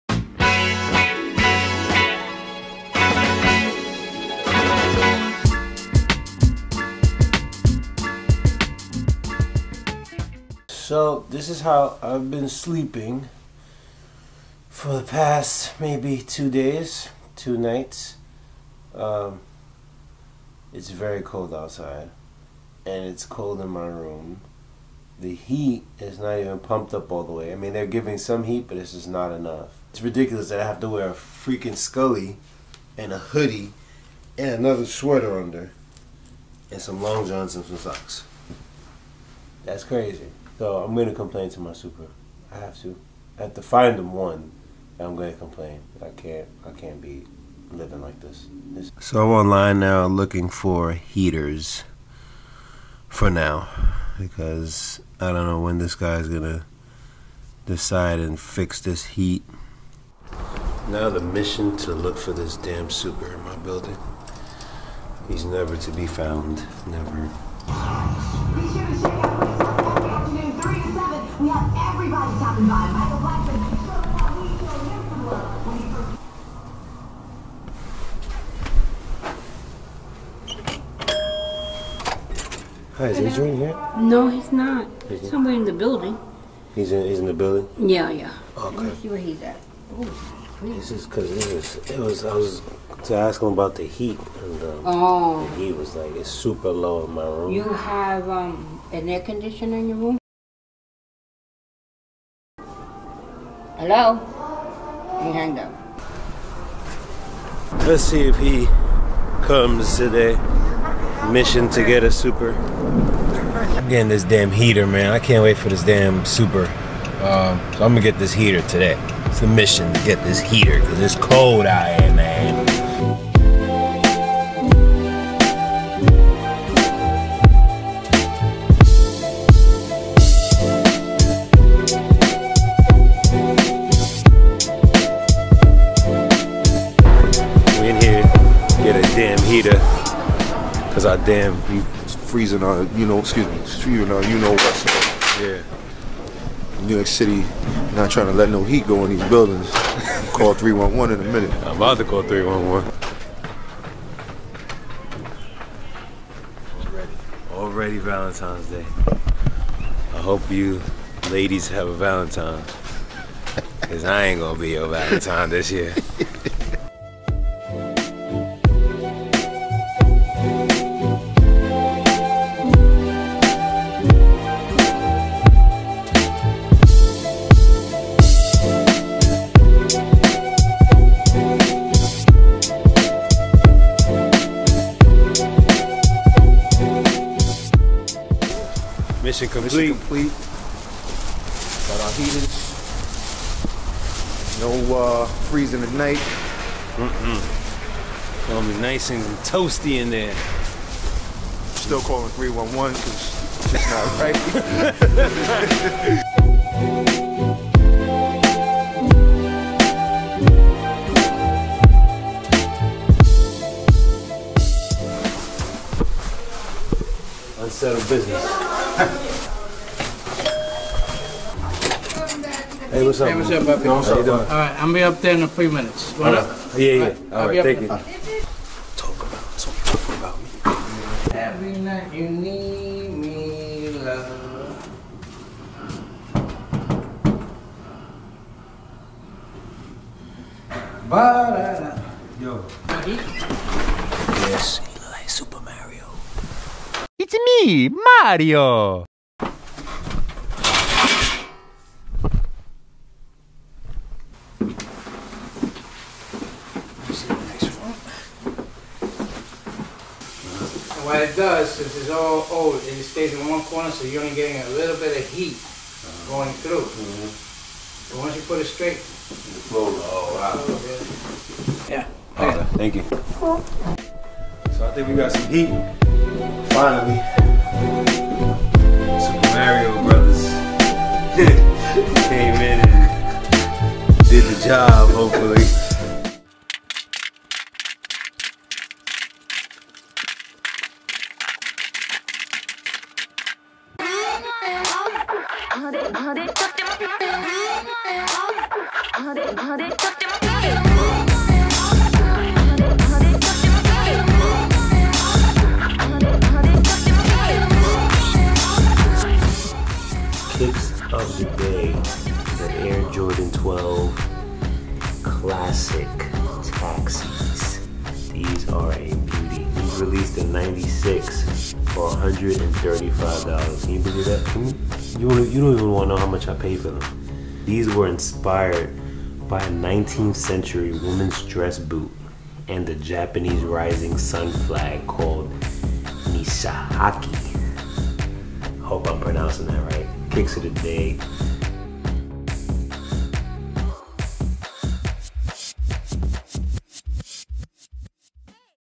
"SUPER" MARIO
This vlog is about NO HEAT IN THIS FREEZING COLD. MISSION TO FIND SUPER AND BUY A HEATER! OH AND KICKS OF THE DAY!

Hit me up!
and on snapchat: JussNYC